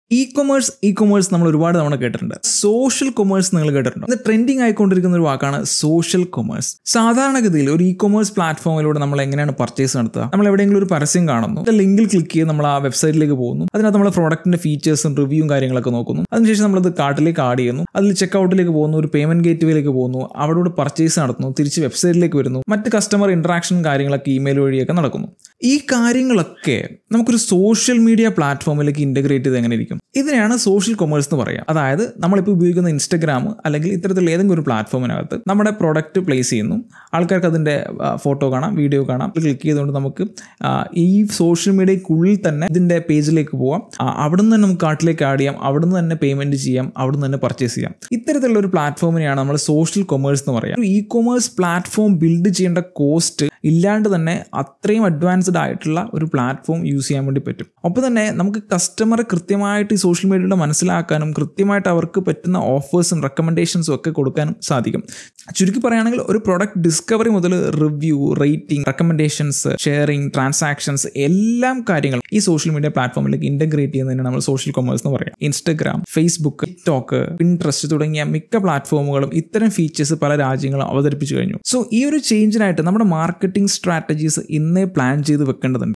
Future is social commerce explained in Malayalam by Jeevan Uthaman
In this video, Mr Jeevan Uthaman explains what is social commerce

for our training programmes, visit brandisam.com or jeevanthetrainer.com

Let us explore the limitless potential of social commerce in Malayalam.